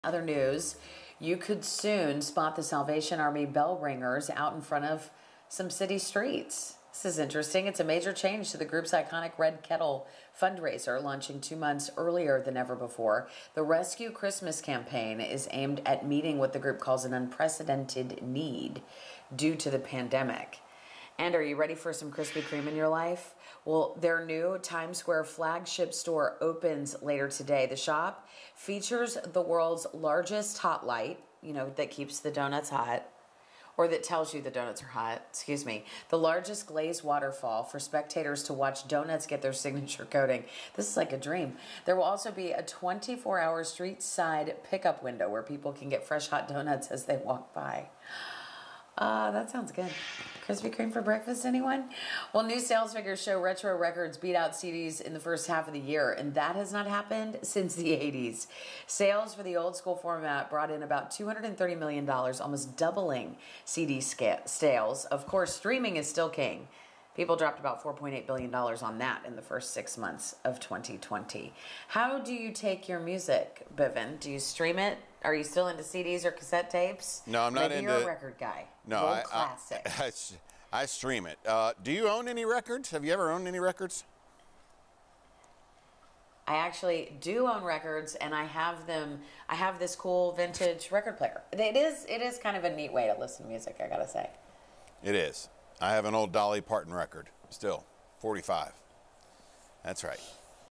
In Other News: Salvation Army starts fundraising early, Krispy Kreme opens NYC Flagship & Vinyls ove
Here are the hot topics "In Other News" for Tuesday, Sept. 15.